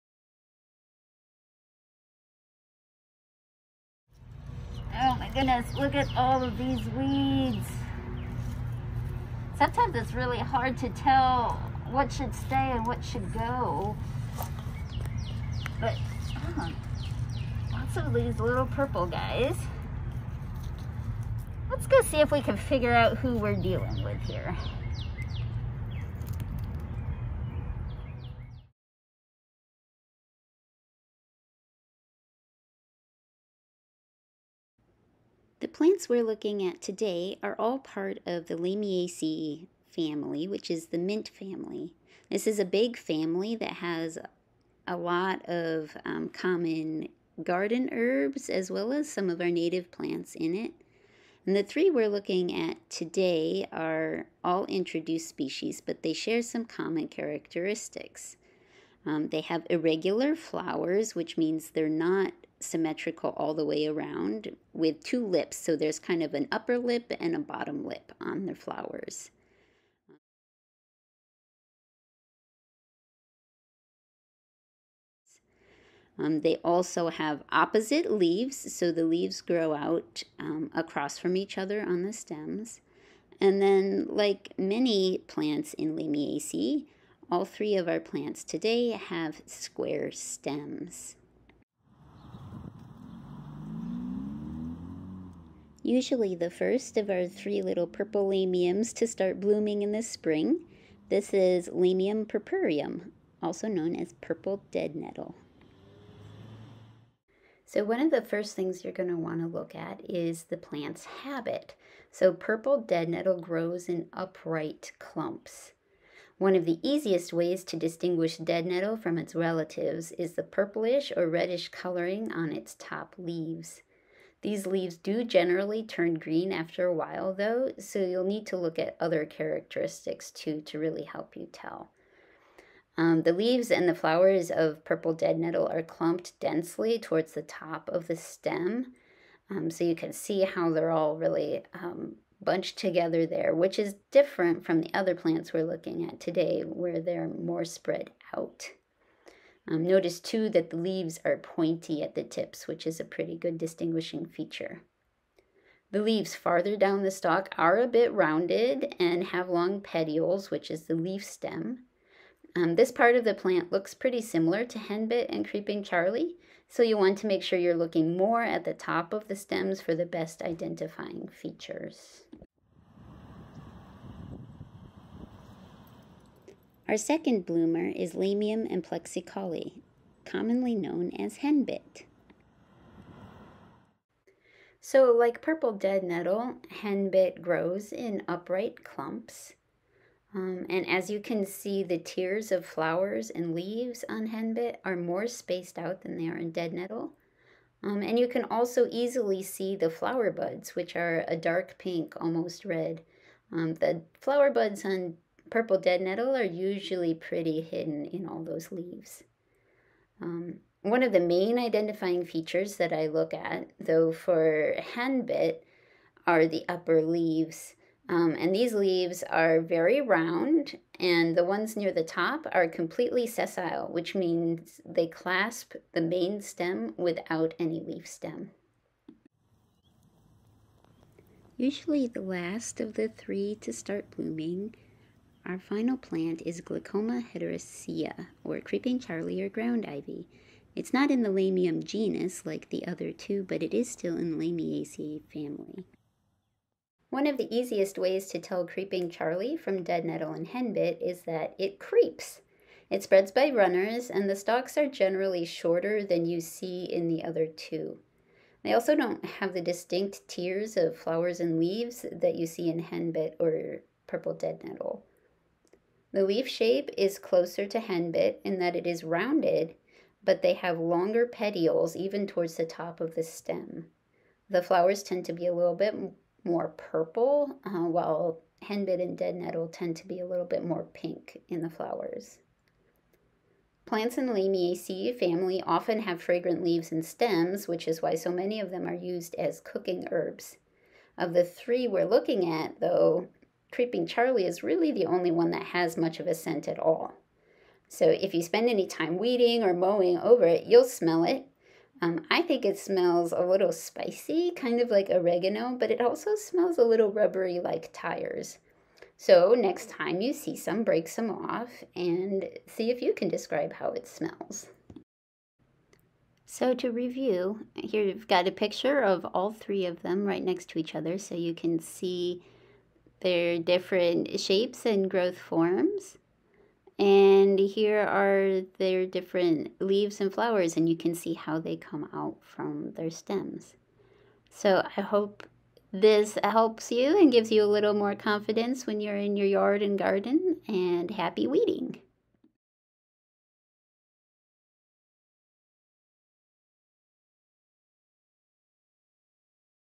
LREC Species Spotlight - Little Purple Lamiaceae Weeds
Want to get rid of those pesky purple exotic weeds growing in your native planting, but you're just not confident in your seedling and weed ID? Well do I have the video for you! Susan Baron describes the the differences between the common purple flowering yard weeds - purple dead nettle, henbit and creeping Charlie/ground ivy in our latest educational video!